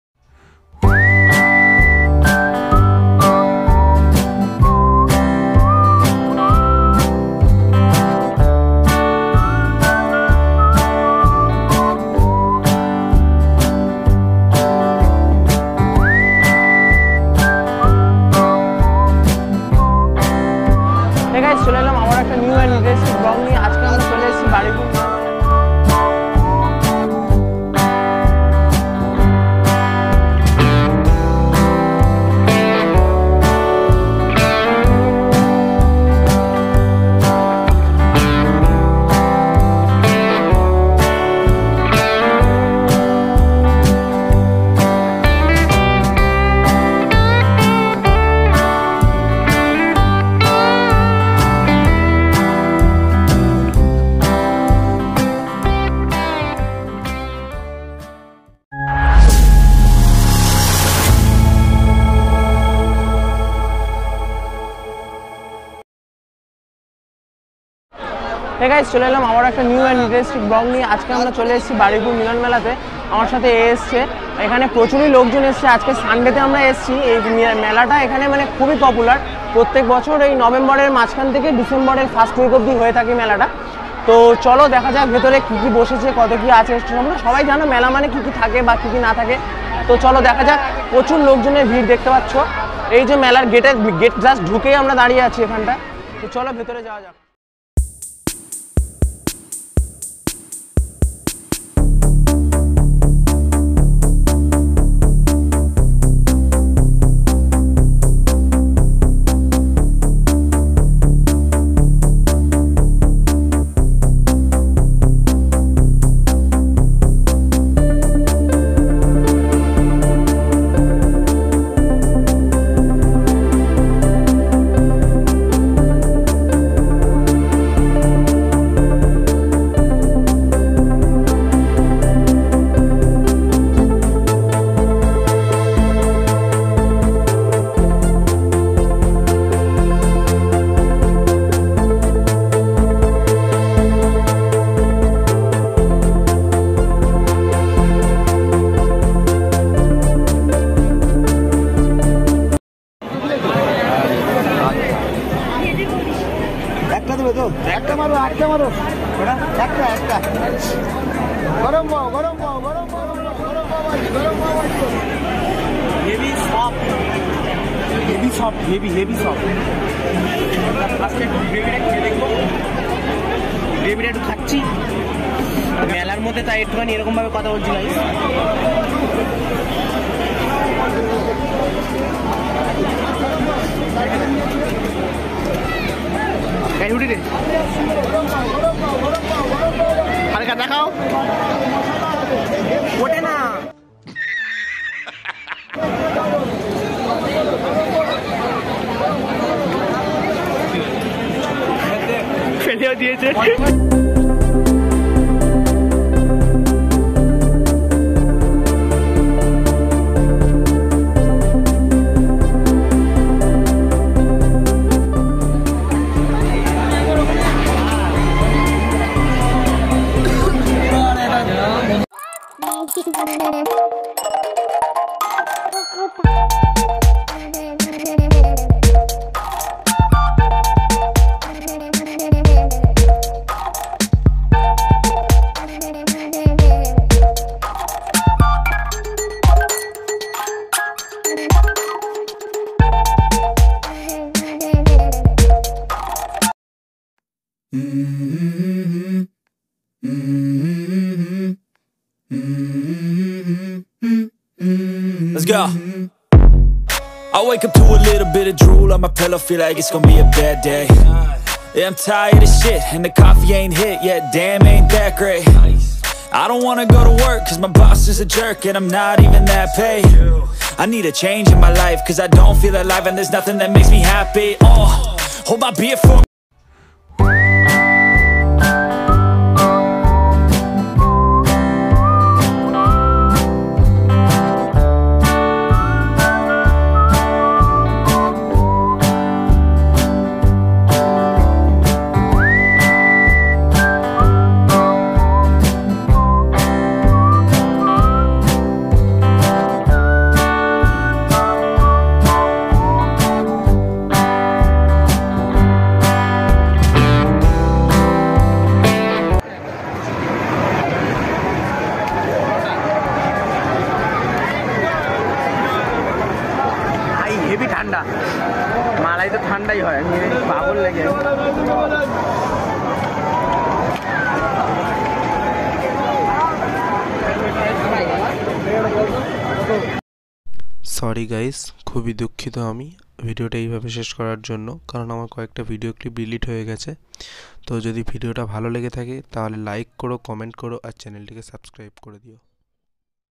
Baruipur Milon Mela 2022,Baruipur Popular mela, NIC Ground Milon Mela
This is a popular fair in baruipur at NIC ground..its happening every year..This fair started from middle of november and end till december 2nd week.Its openning time is 03:00 pm. and  close time is 10:00-11:00 pm.

VENUE;
Milon mela
NIC Ground
baruipur

baruipur town.